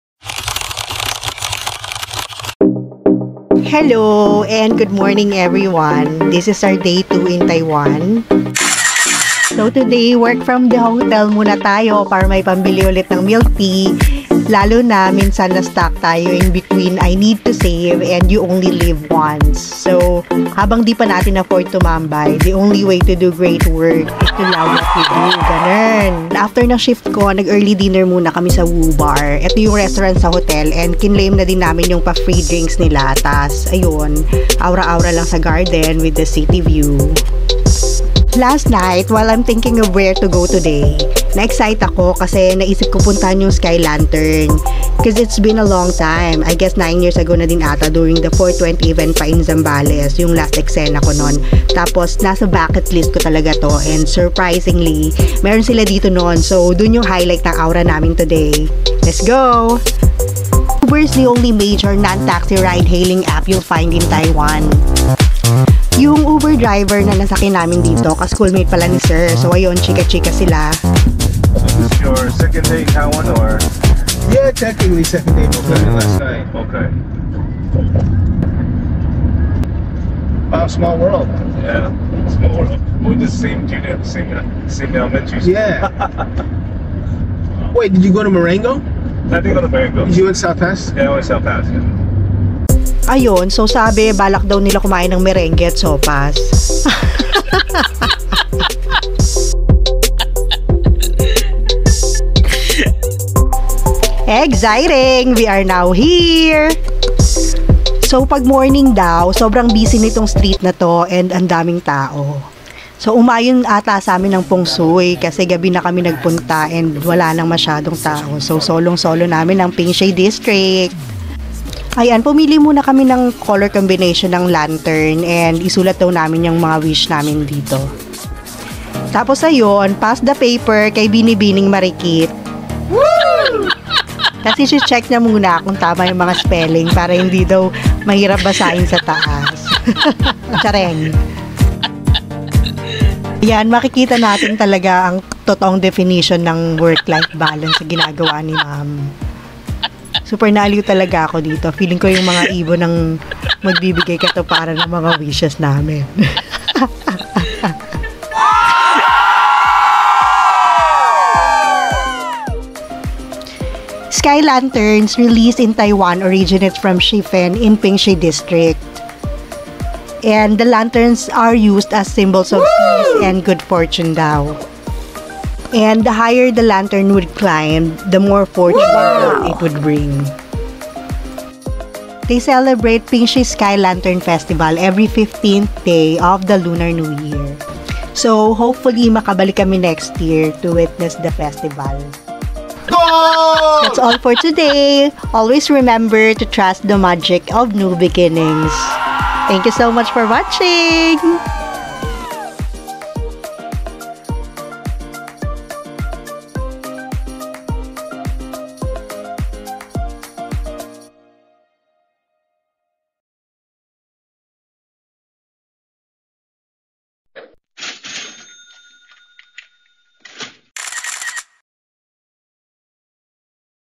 TAIWAN DAY 2/5: WORK FROM THE HOTEL | WOO BAR | SHIFEN SKY LANTERN | DIY TOUR
In this video will be sharing the sky lantern experience.